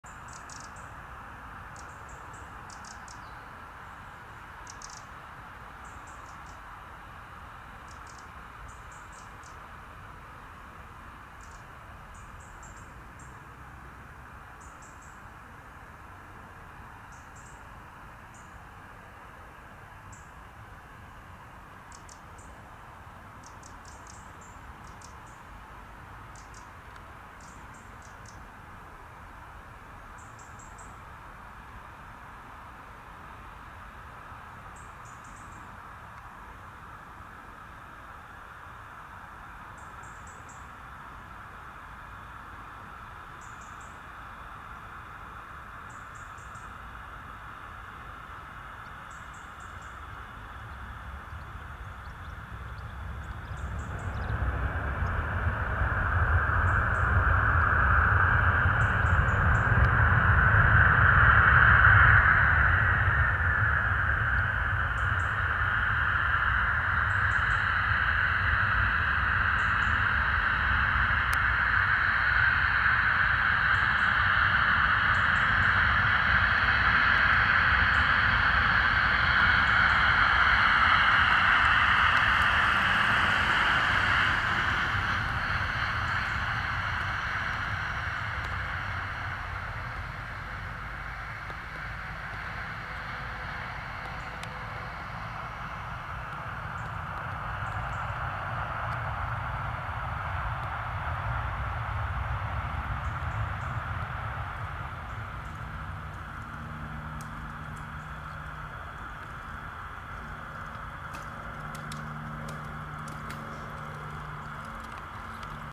SUNSET ARRIVAL - GLOBAL EXPRESS - TC-TSS LANDING AT FARNBOROUGH-EGLF UK 17-11-2023
SUNSET ARRIVAL AT FARNBOROUGH - GLOBAL EXPRESS TC-TSS LANDING ON RUNWAY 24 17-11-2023